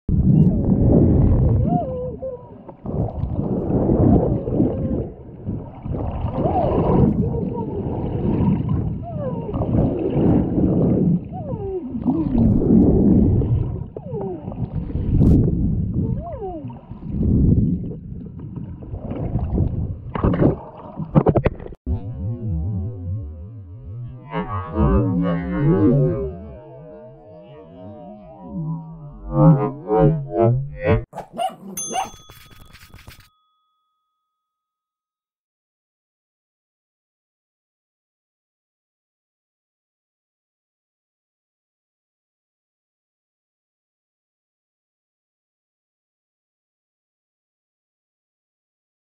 Swimmer has near miss with torpedoing sailfish
Tonga is a beautiful area of the world, and one of the few places where people can get into the water with humpback whales. These gentle giants travel here in July from all around the world so that they can breed and bear their young in waters where there are no orcas or large sharks. Humpbacks have few predators when they are full grown, but the calves are vulnerable to these predators when they are small. Swimmers were filming this incredible animal near the island of Vava'u, enjoying a spectacular display as the humpback rolled at the surface. But a blur of motion caught the eye of the swimmer with the camera as she they finished watching the whale. Too fast to be seen clearly, the swimmer was only aware of the fact that something large and incredibly fast had passed close by. Watching the footage later and slowing it down revealed that a large sailfish had passed between the whale and the swimmer. Sailfish are the fastest swimmers in the ocean. Their top speed is the topic of debate, with some sources citing that they can reach 125km/h (68mph), while others say that 35km/h is the highest recorded speed. They use this great speed to spear and injure fish with their long and sharp bill. Impaling or slashing fish causes them to swim much slower, allowing the sailfish to capture them. This is a particularly effective means of attack when more than one sailfish attacks a school of smaller fish, such as sardines. This swimmer was not in any serious danger, but the thought of such a large fish coming past like a torpedo with a harpoon on the front was intimidating. It is rare for a swimmer to catch sight of a sailfish as they will avoid humans, and it is even more rare for a person to capture an event like this on video.